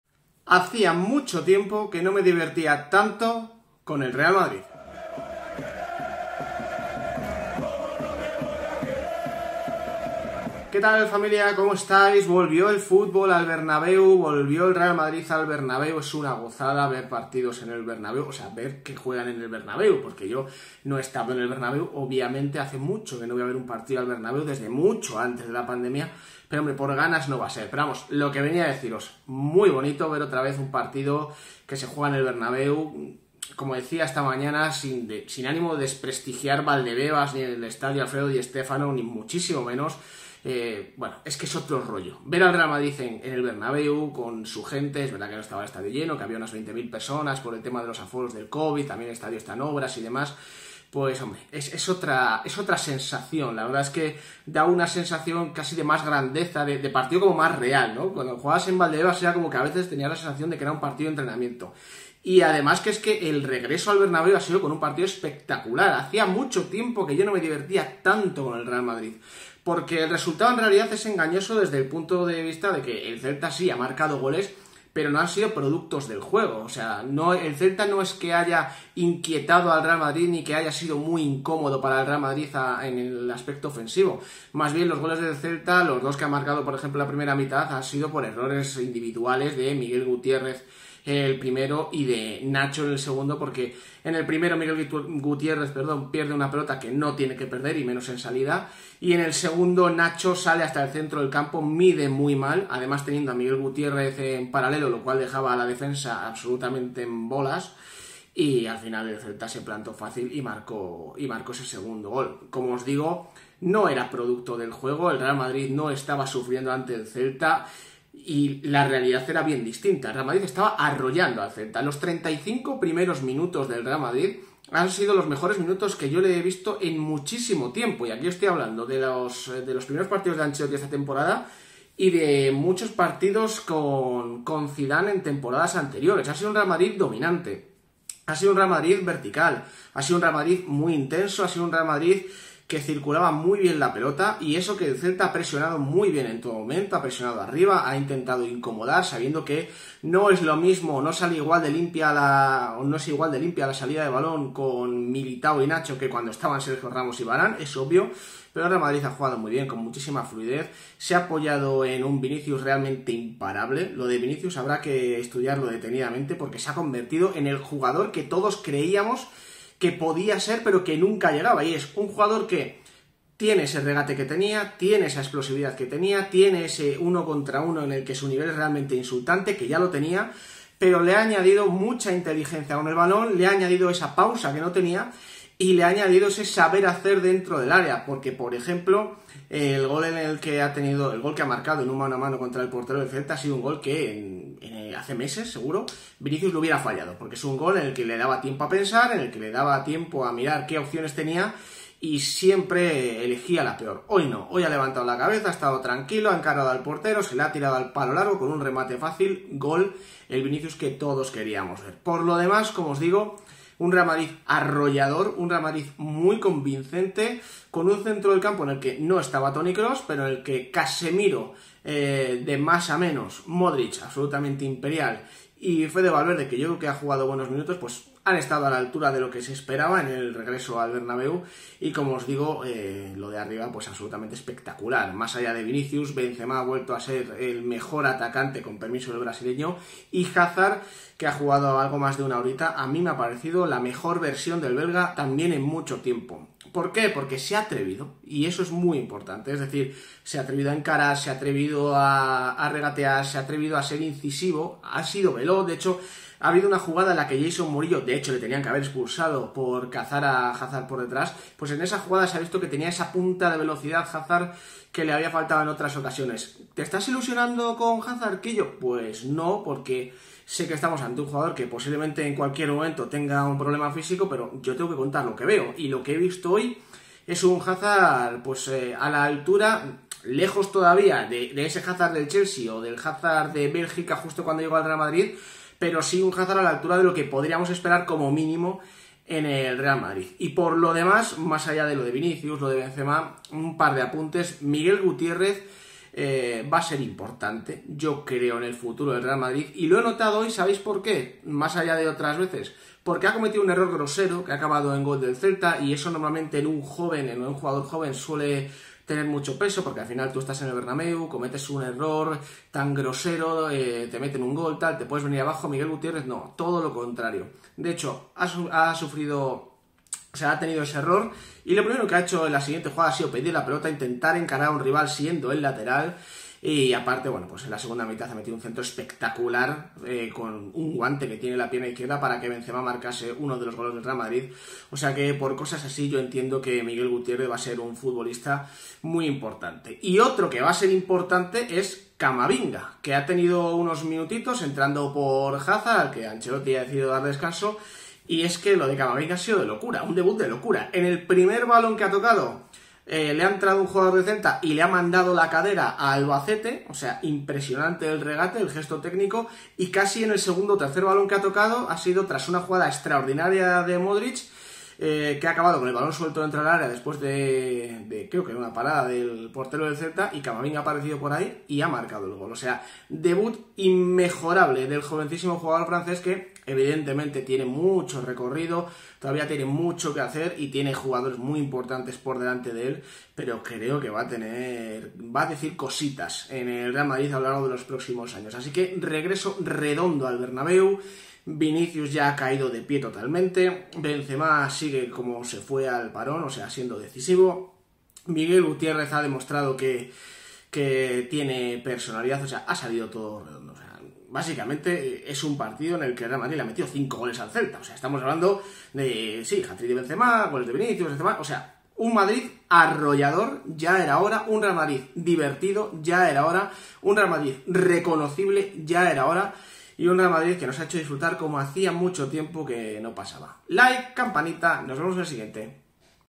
REAL MADRID 5-2 CELTA: BENZEMA Y VINICIUS LIDERAN LA TORMENTA PERFECTA | REGRESO SOÑADO AL BERNABÉU
Karim Benzema y Vinicius lo bordaron ante el Celta en el regreso del Real Madrid al Bernabéu. El francés marcó un hat trick y el brasileño destrozó a la defensa celeste. Debutó Camavinga y lo hizo con gol. Por fin nos divertimos con el equipo blanco.